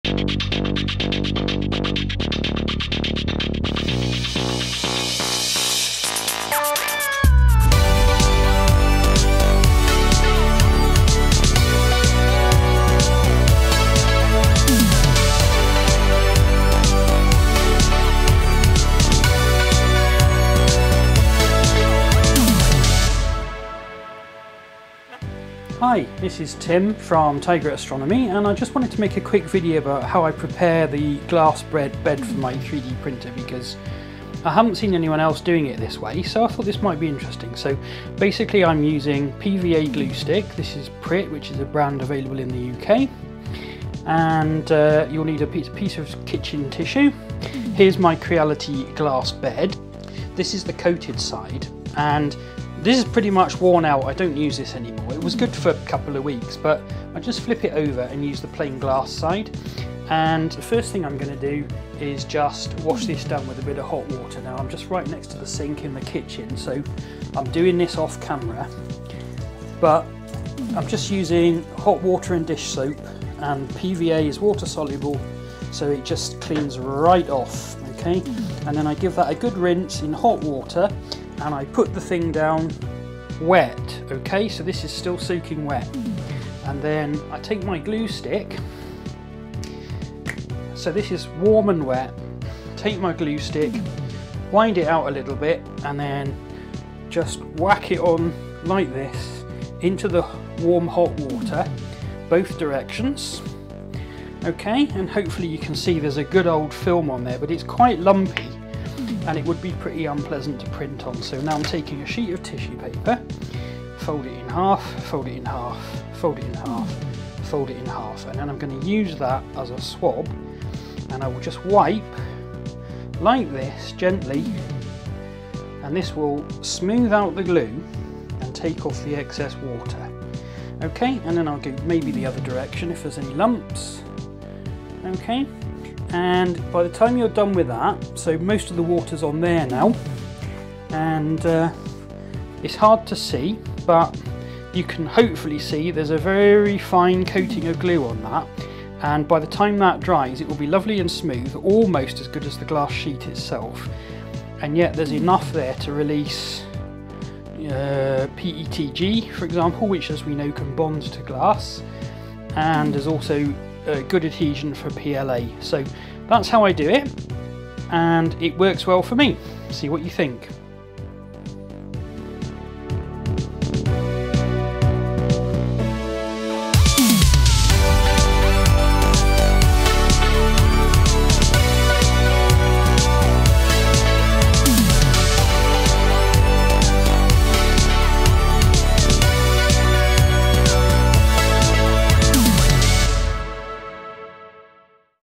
Preparing a Glass Bed for 3D Printing with PVA Glue Stick
My technique for preparing my glass print bed ready for 3D printing with PLA, PLA+, SPLA and PETG.